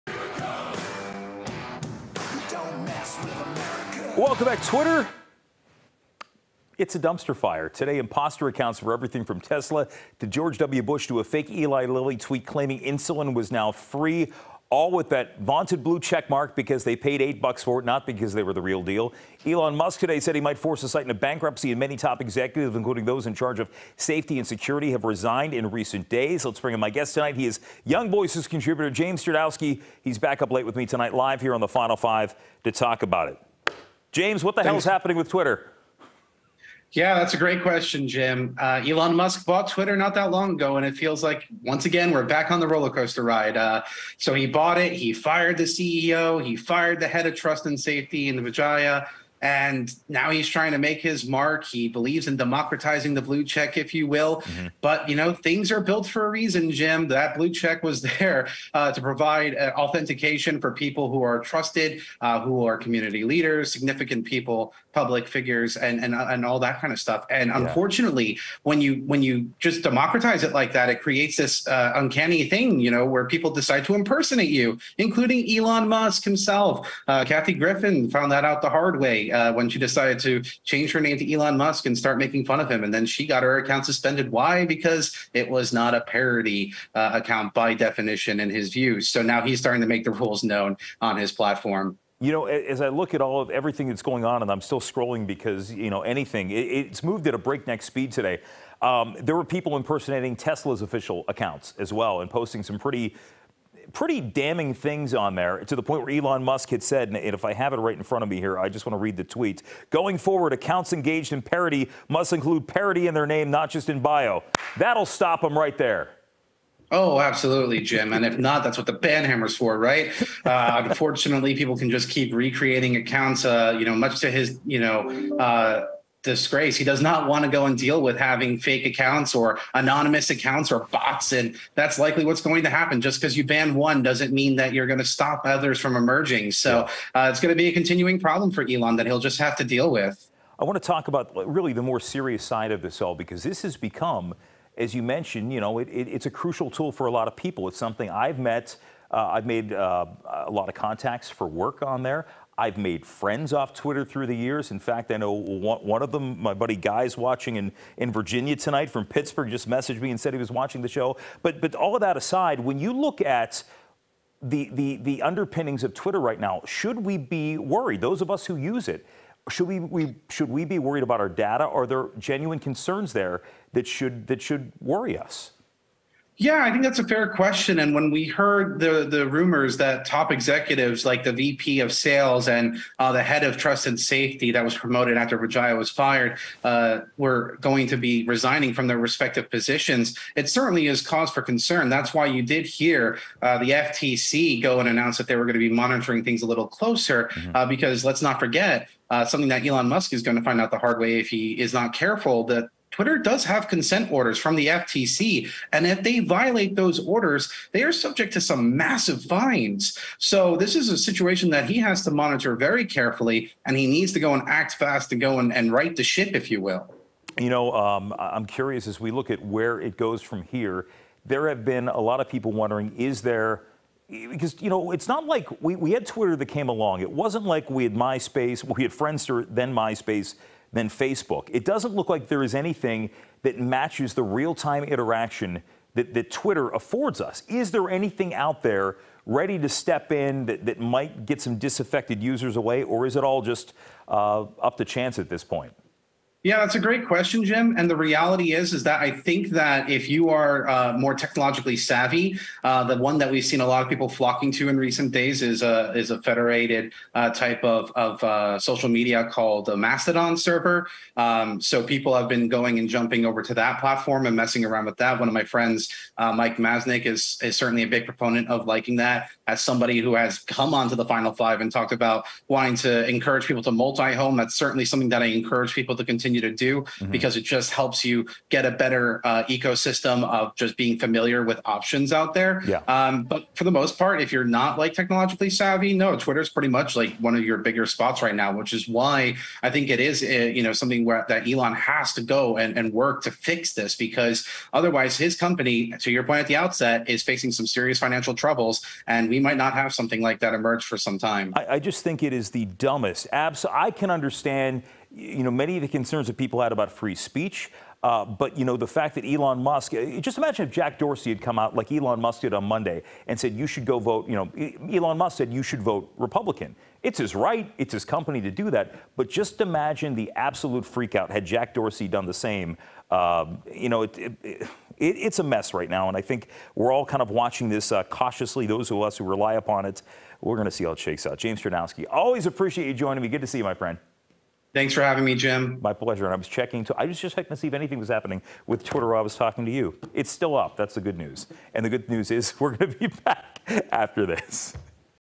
TheFinal5: Will Twitter survive Musk?
Thursday brought a breakneck series of events at Elon Musk's Twitter. Young Voices contributor James Czerniawski joins Jim to discuss.